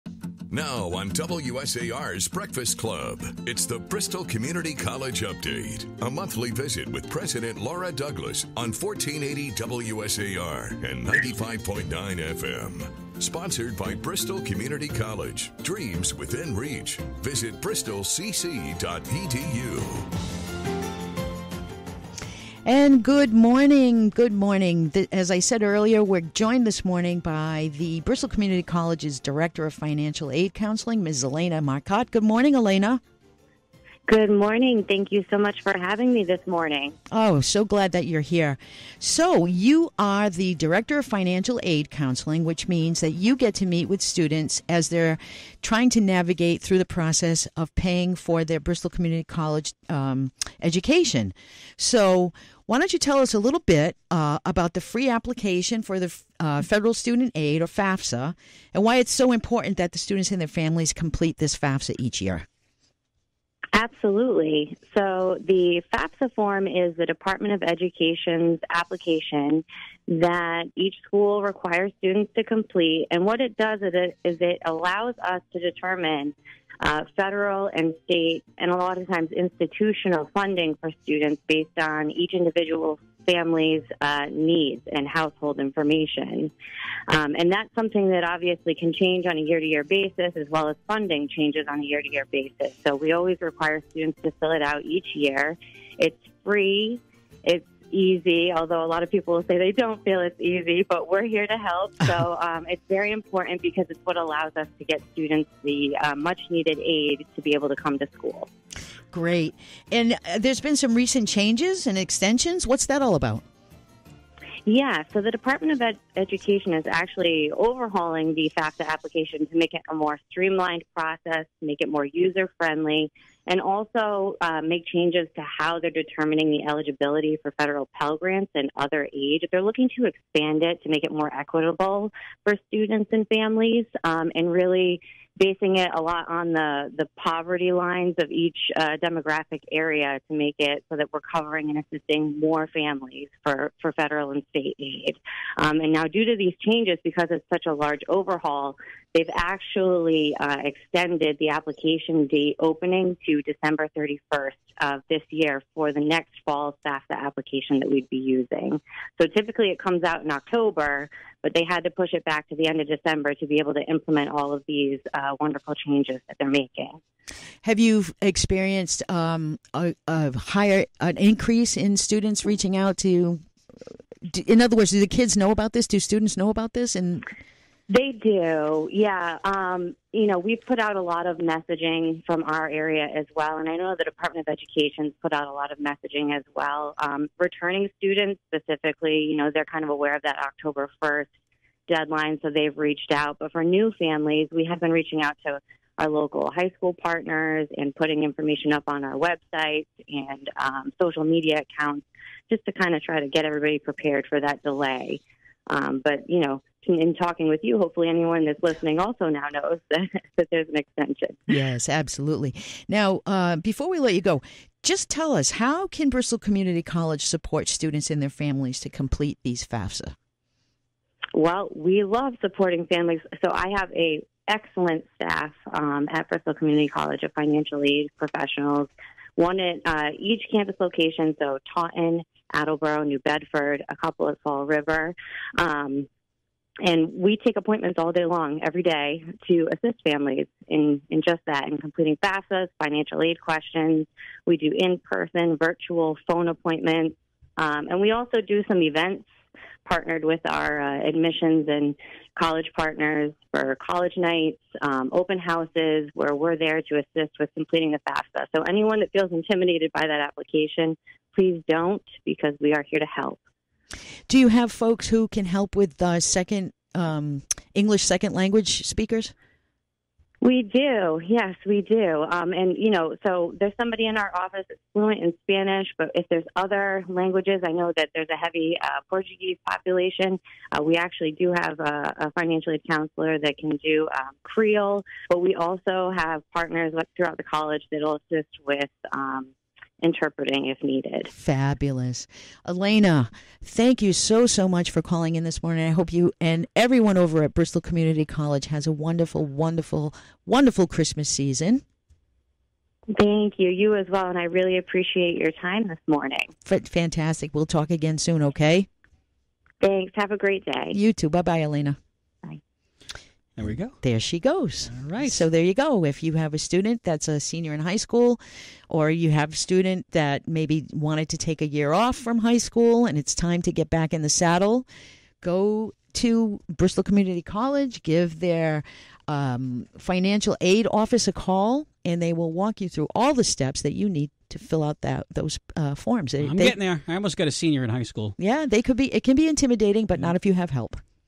Bristol Community College Update on WSAR: December 2023
Bristol Community College Update on WSAR: Free Application for Federal Student Aid (FAFSA)

Bristol Community College Director of Financial Aid Counseling Alaina Marcotte discusses the Free Application for Federal Student Aid (FAFSA) and why it is important for students and their families to complete the FAFSA. Alaina also shares information about the recent changes to the FAFSA and how Bristol can support students in completing the application.